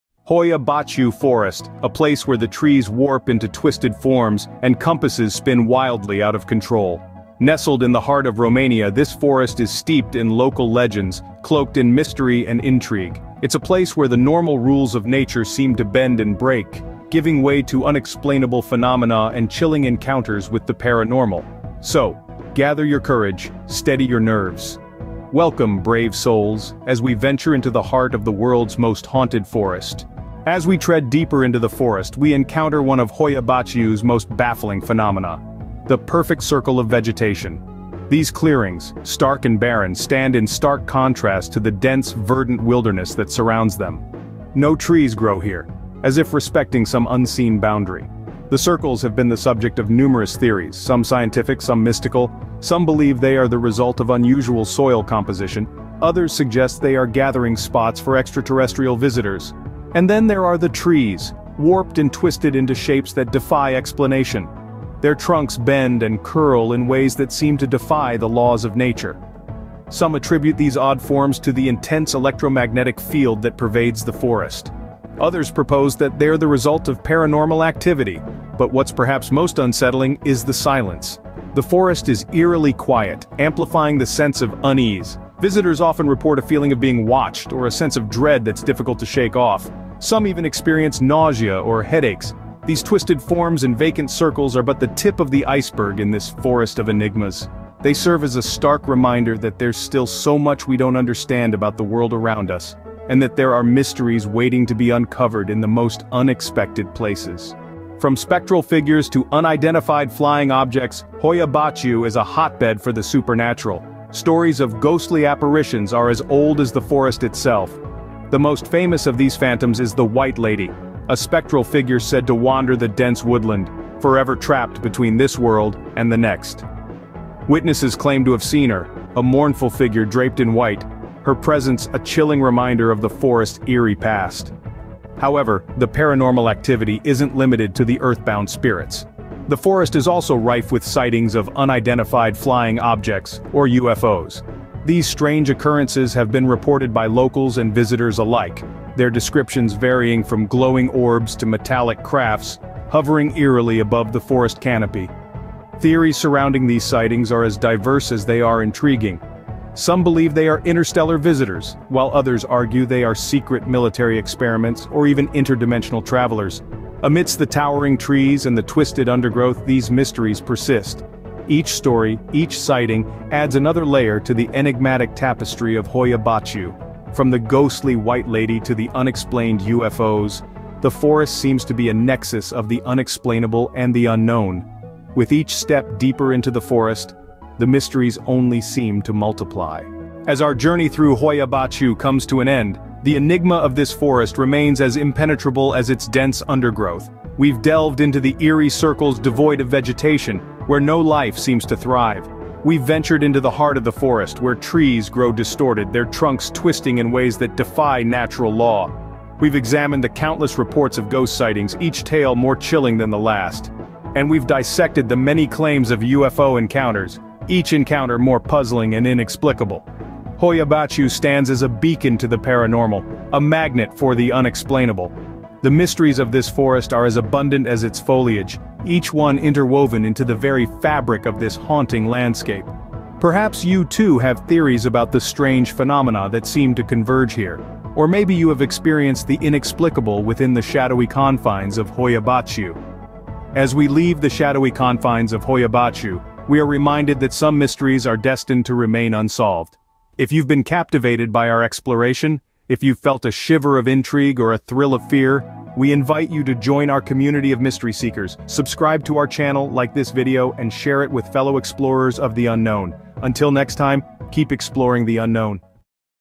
Explore the Mysteries of World's Most Haunted Forest HOIYA BACIU!
Hoia Baciu… this vast ancient woodland has an extensive history of strange happenings stretching way back through the years, gaining it recognition as the most haunted forest in the world but what begins as a haunting, morphs into something much stranger…
Brave visitors now peer into the sky for answers.

People say this forest is a gateway to different realms, some say its related to alien visitors, others say theres a magnetic force within the ground of the forest.

After so many strange stories, is it all make believe, or is there really something going on in this Transylvanian wood? After our visit, we think its really no wonder its been named the worlds most haunted forest.

Hoia Baciu Forest (Romania): Often called the "Bermuda Triangle of Transylvania," this forest is known for its unusual paranormal activity, including reports of UFO sightings, unexplained disappearances, and feelings of anxiety or nausea among visitors.